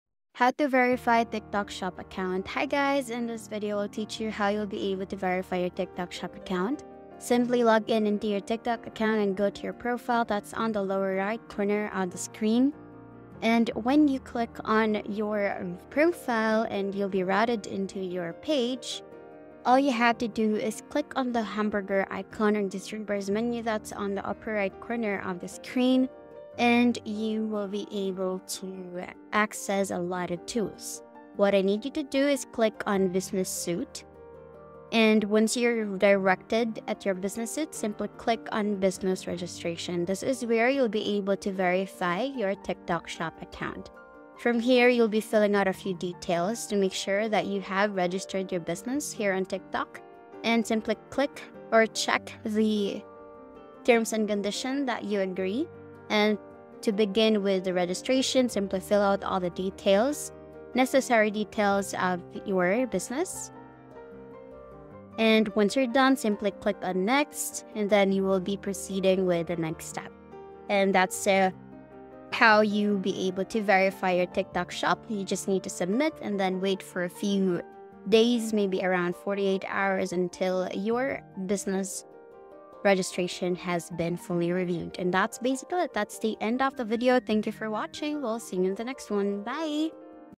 How to Verify Tiktok Shop Account (Best Method)
From setting up your shop to completing the verification process, we'll guide you step by step to ensure your account is verified and ready to start selling! Don't miss out on this helpful tutorial for online sellers.